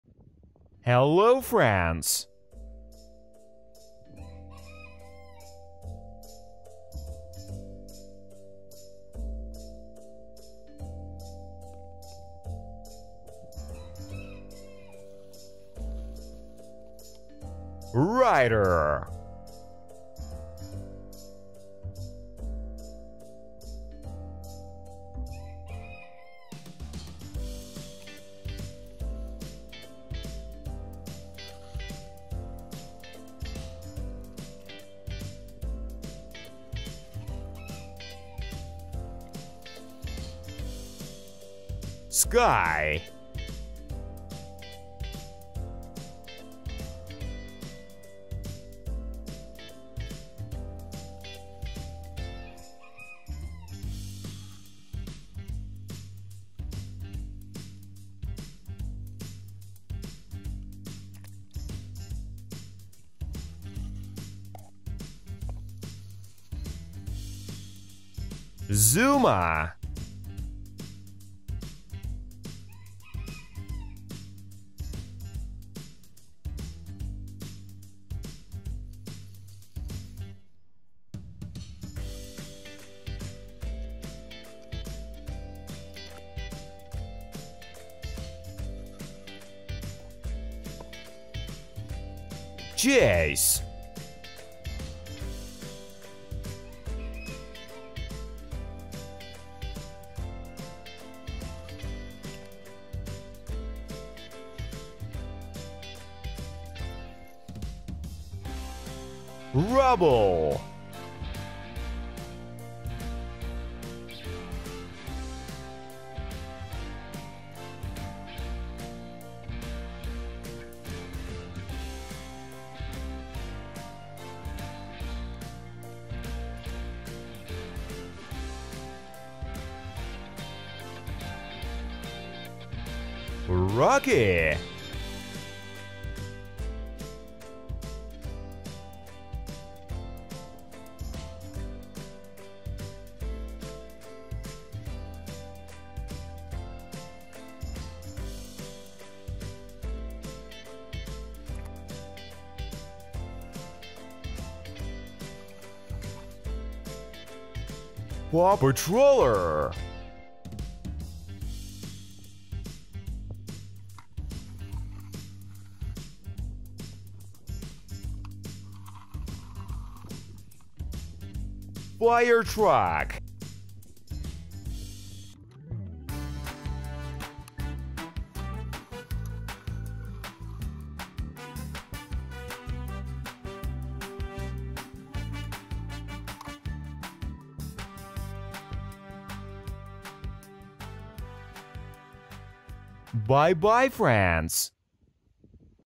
Looking PAW Patrol | PAW Patrol Toys | Heroes: Fire Truck, Skye, Marshall, Rubble| Video for kids
Paw Patrol is a popular children's toy line and animated TV series that features a team of heroic rescue dogs who go on exciting missions to protect their community. In this particular set, the Paw Patrol toys have arrived at the beach for some fun in the sun and ocean adventures.

Each toy represents a member of the Paw Patrol team, including Chase the police dog, Marshall the firefighting dog, Skye the aviation dog, and others. These toys are made of durable metaland are painted with bright colors and accurate character details, making them instantly recognizable to fans of the show.